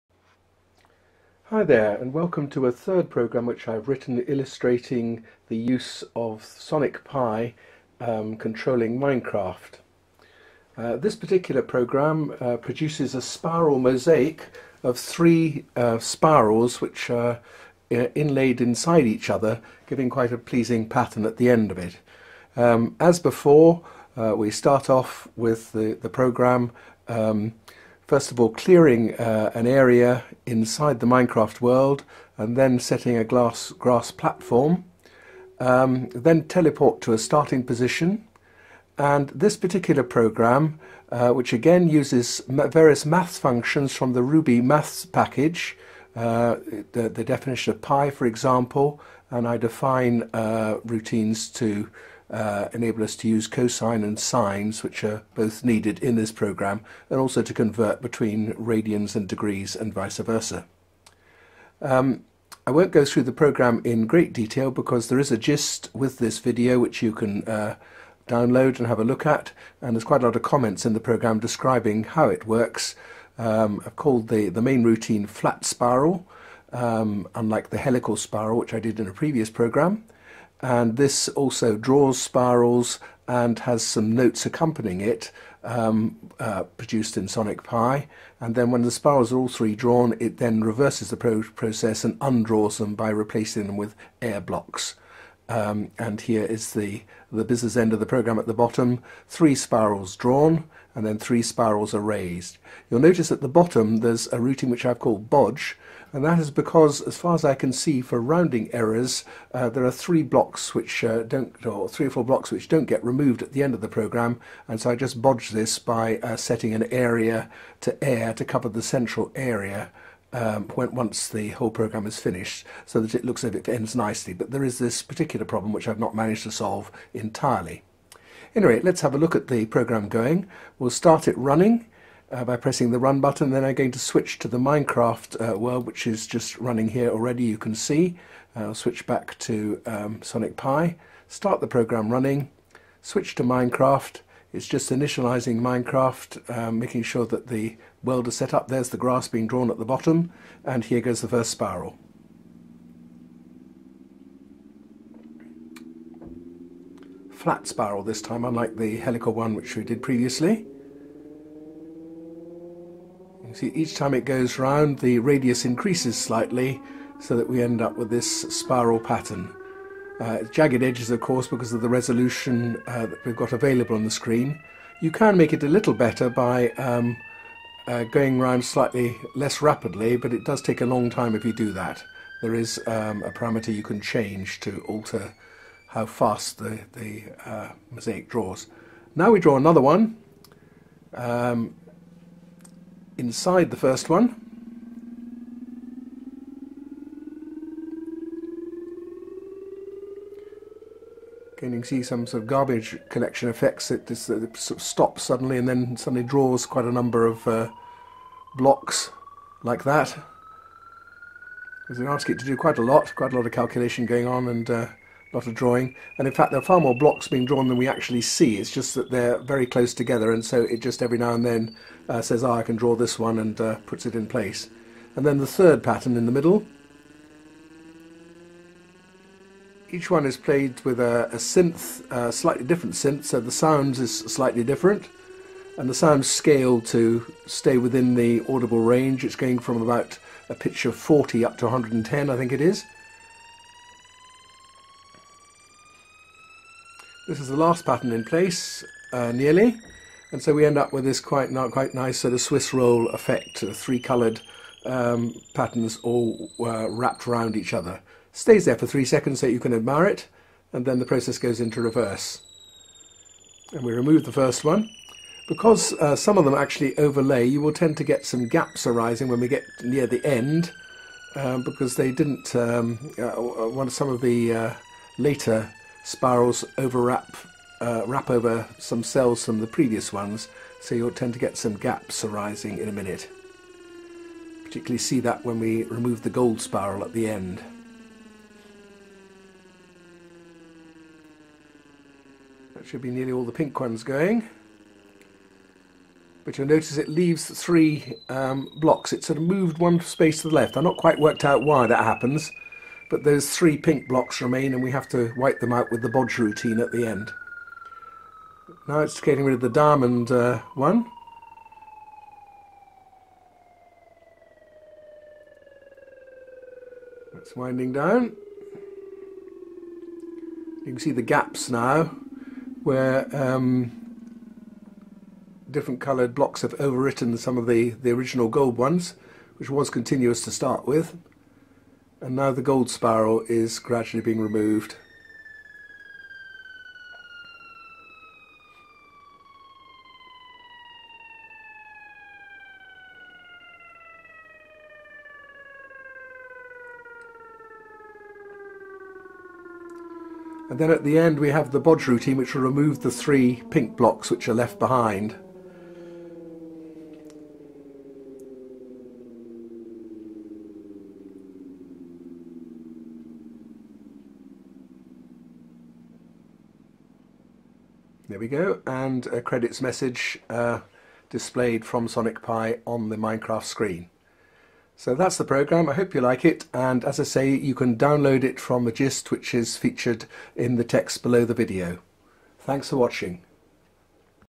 SonicPiMinecraftMosaicSpirals
Sonic Pi Minecraft API generates a mosaic of spirals with musical accompaniment. Produced with version 2.5dev. The syntax MAY alter with the release version 2.5 due shortly.